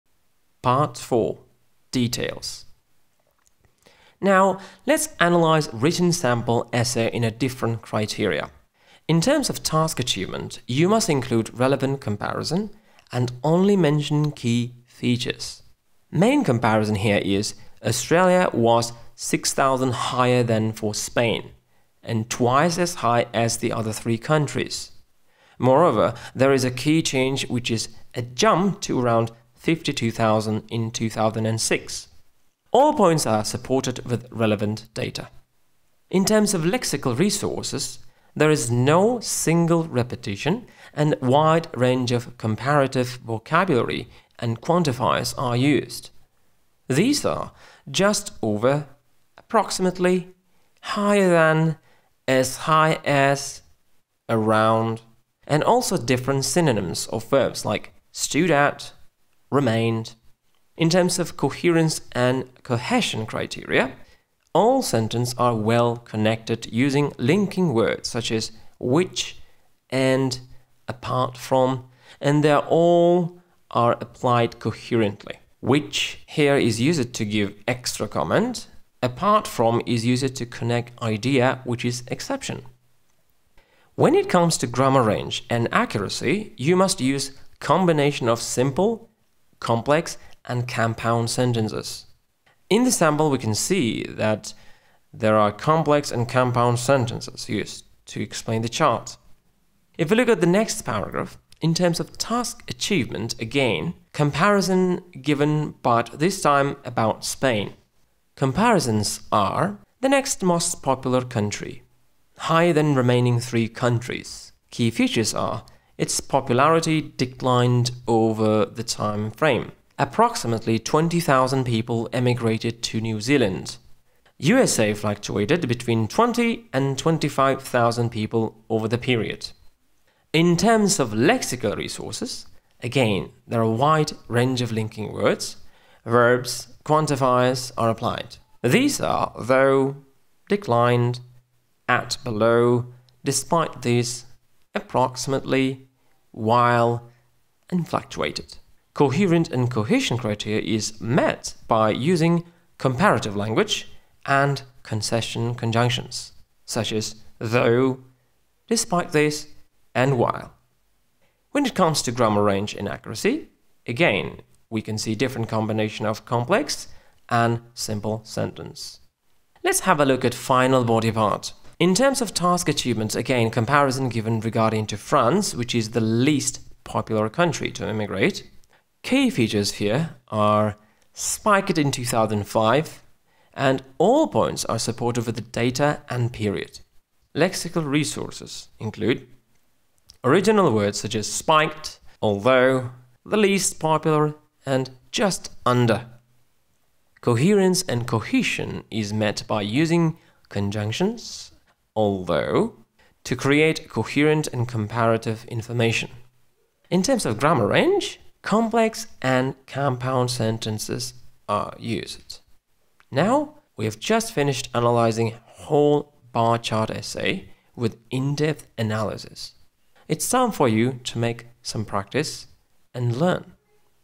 Bar chart (whole essay) part 3
Ushbu videoda siz IELTS writing task1 bar chart inshosini yozishni o'rganib olasiz.
In this essay, you can learn how to IELTS write task 1 bar chart essay.